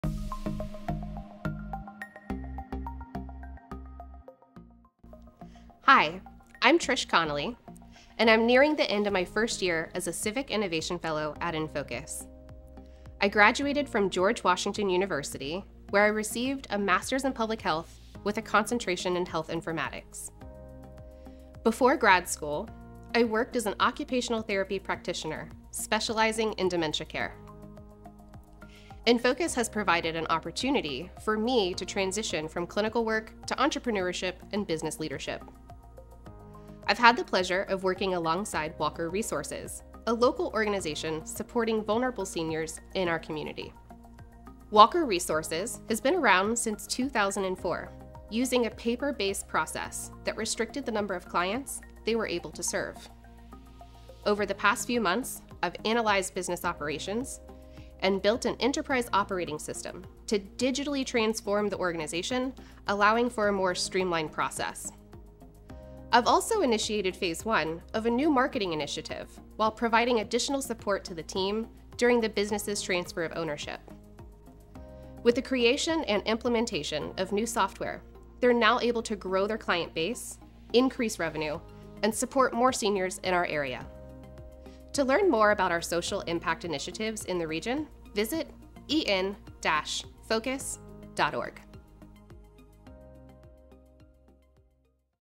Protecting Seniors in Northern Indiana
Trish Connelly is an enFocus Fellow preparing for a business ownership transition of Walker Resources. Since 2004, Walker Resources LLC aims to improve protections for seniors. Walker Resources LLC works collaboratively with family members, judges, attorneys, realtors, and health providers to reduce financial exploitation and elder abuse.